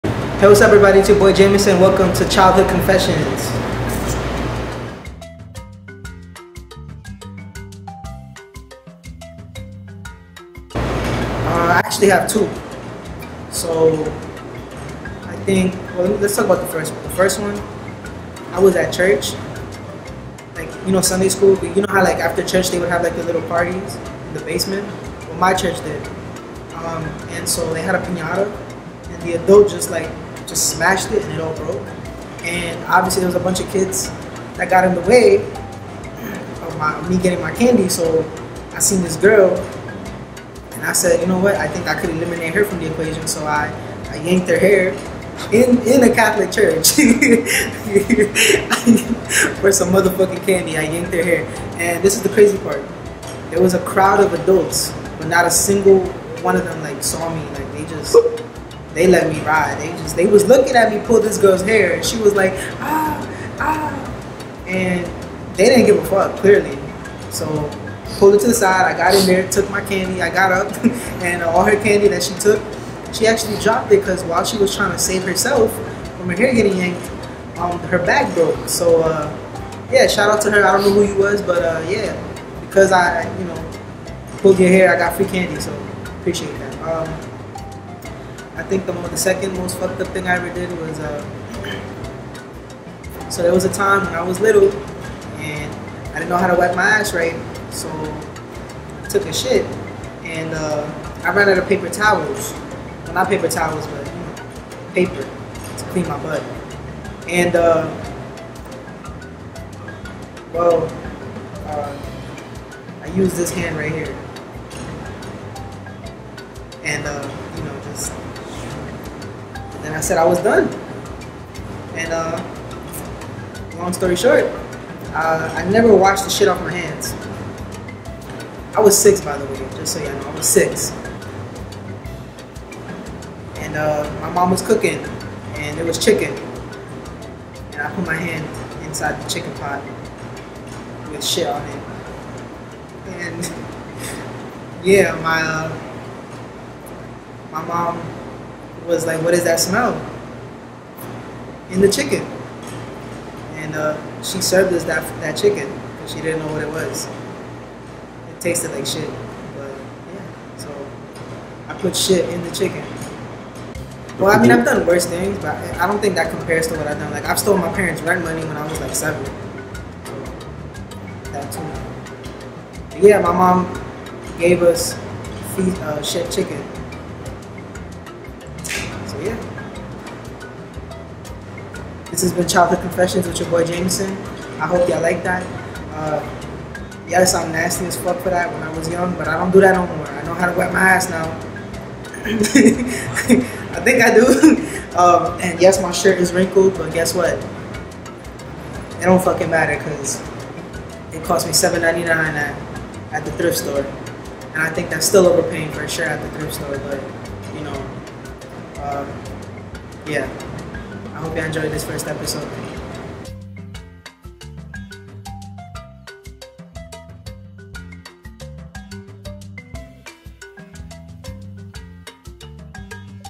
How Disgusting Jameson Was As A Kid | Childhood Confessions | Gentrified Idiots
My name is Jameson and I am doing dumb things for my ENTERTAINMENT and yours lol 😂